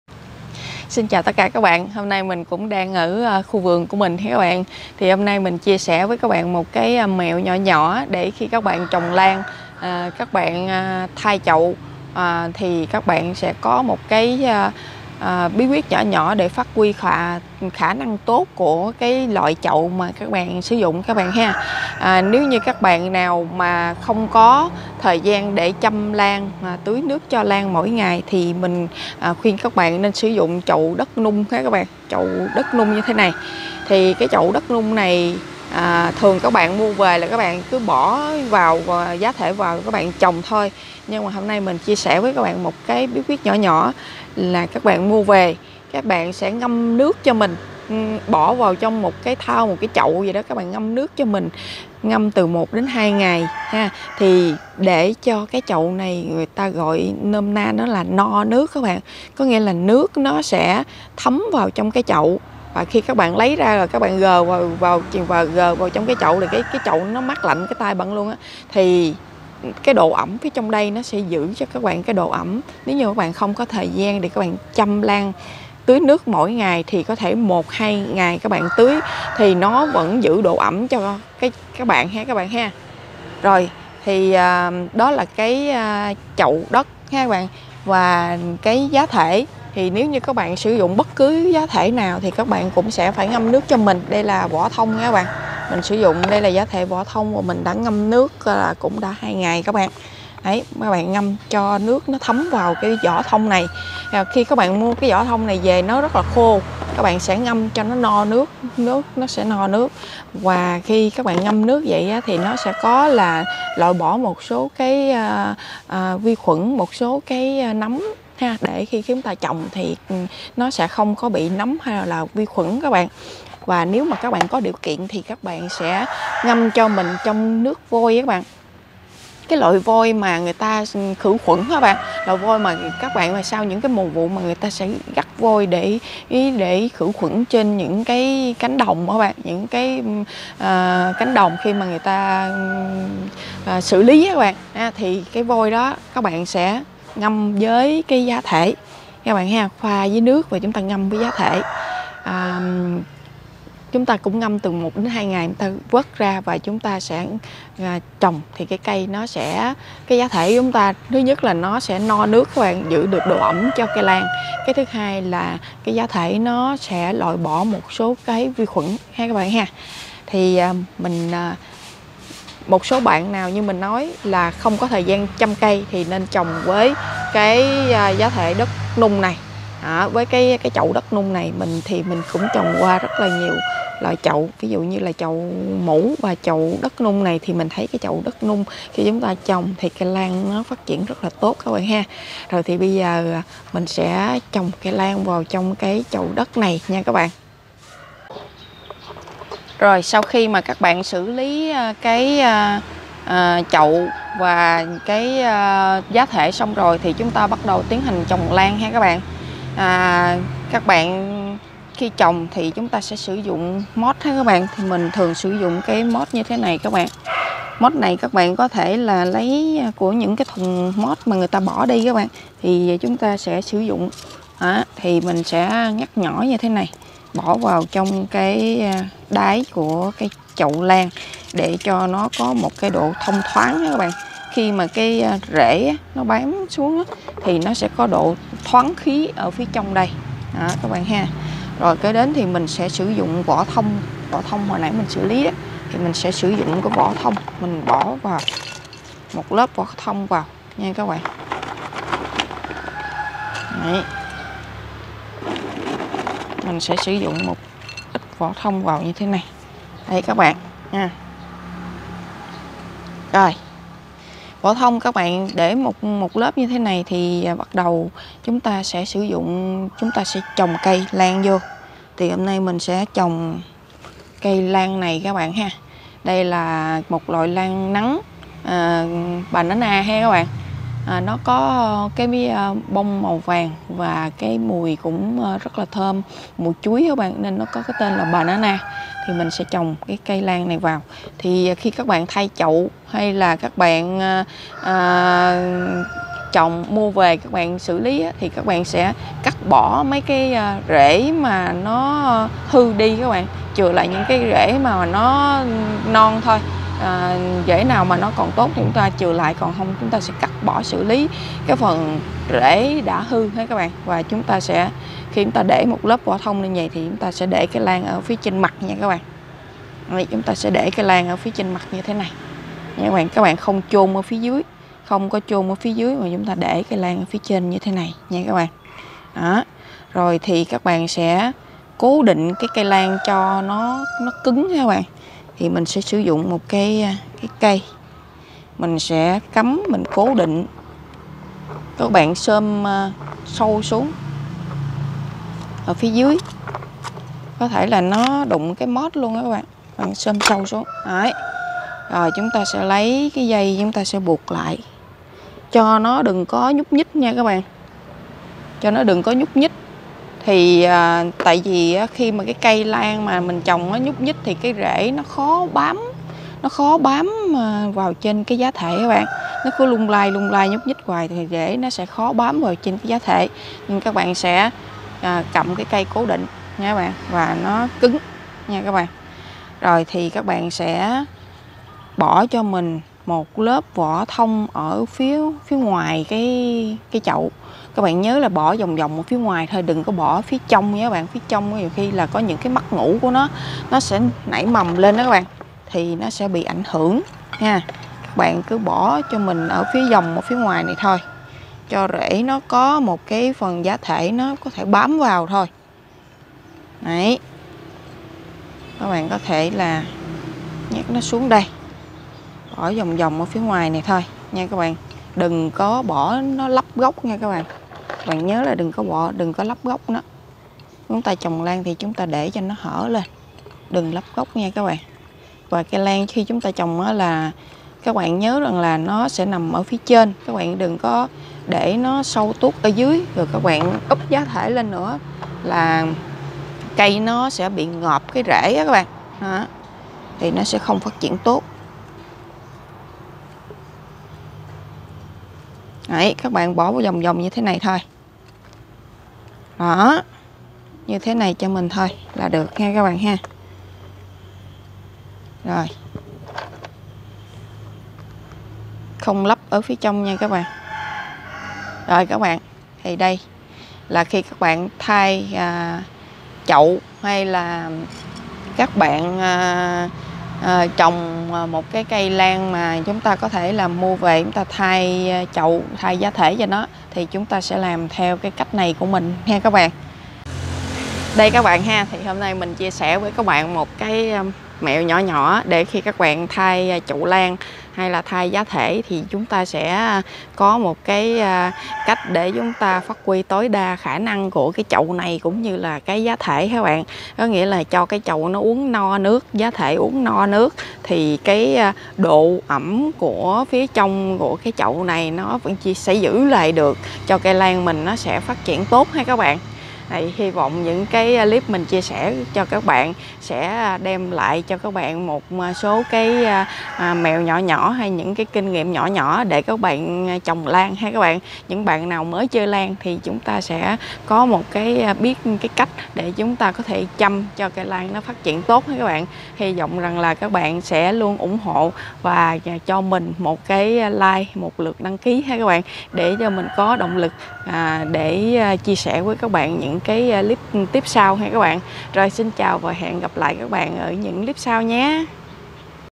Cho chậu đất nung và giá thể uống no nước trước khi trồng lan.
Cây lan cần độ ẩm cho bộ rể phát triển nên cần cho chậu và giá thể uống đầy nước.